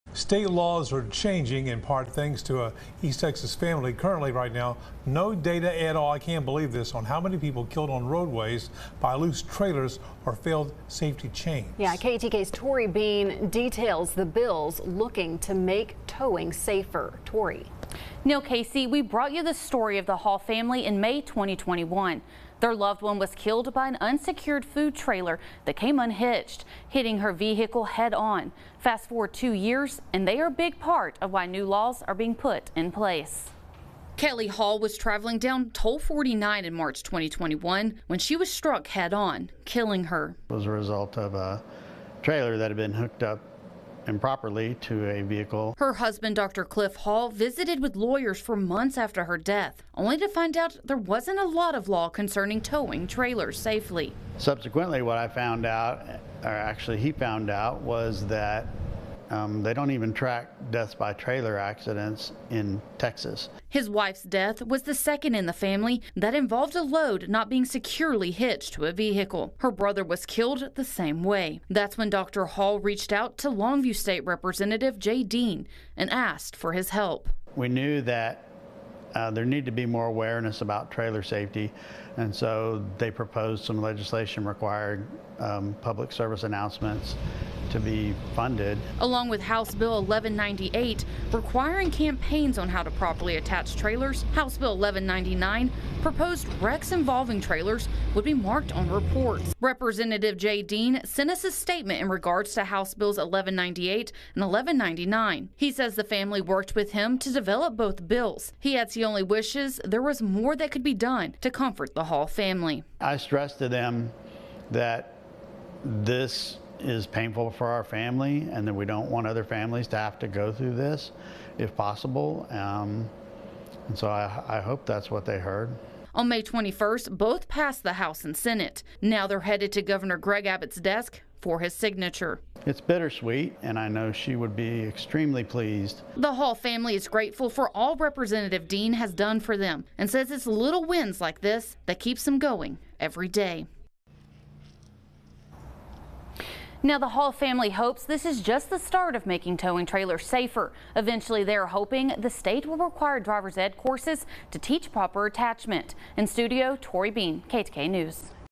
New trailer safety bills moving to Gov. Abbott's desk
KETK brought you the story of the Hall family in May 2021. Fast forward two years and her family is a big part of why new laws are being put in place.